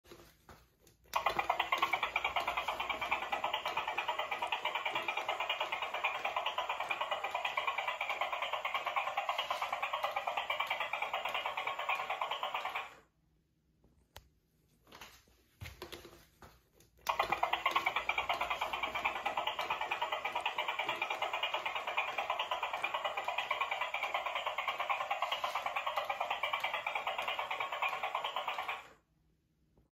Gemmy (NEW for) 2023 Animated Crawling Bloody Hand
THANK YOU Gemmy for finally changing your crawling hands from latex to soft PVC. I don’t know how it took this long, but better late than never I suppose. Made by Gemmy Industries, new for 2023, exclusively at At Home.